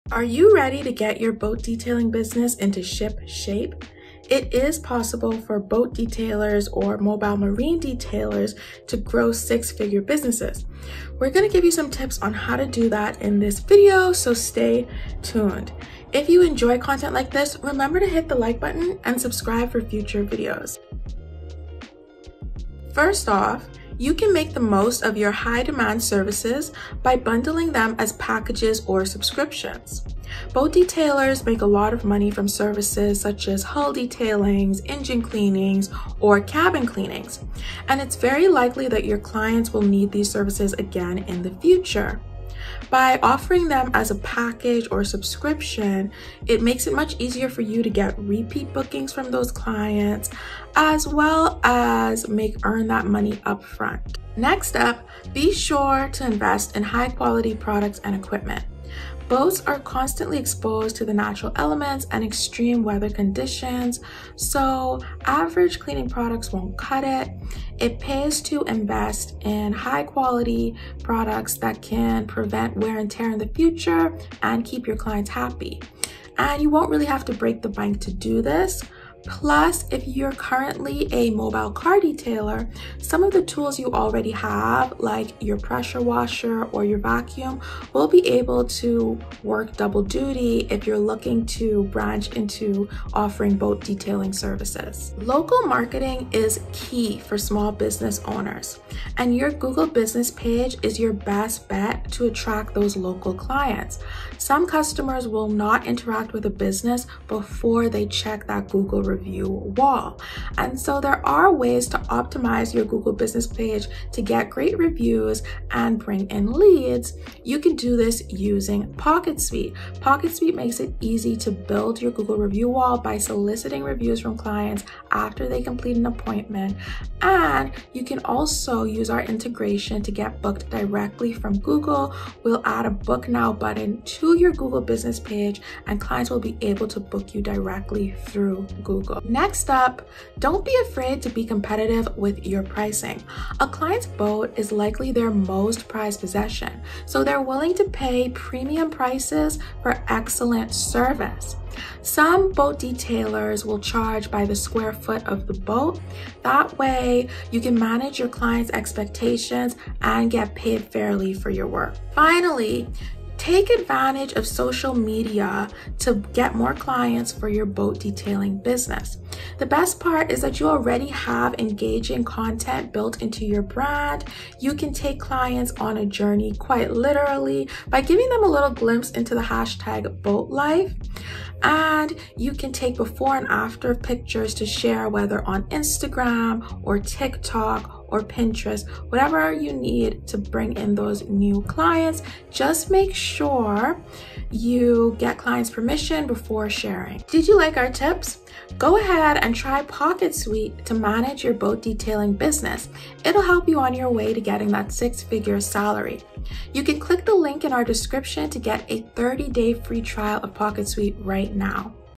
Use these BOAT DETAILING business tips to make SIX-FIGURES
Are you ready to get your boat detailing business into shipshape? As you’re cleaning, waxing, and sanding, be sure to think of ways to expand your client base and grow your income. Mobile marine detailers can hit a six-figure income with time, effort, and a few tried and true strategies.

Read this article, we give your experts tips to grow a successful boat detailing business

00:00 Intro
00:23 Service Bundles
00:54 Equipment
01:35 Leads and Reviews
02:19 Pricing
02:44 Social Media Content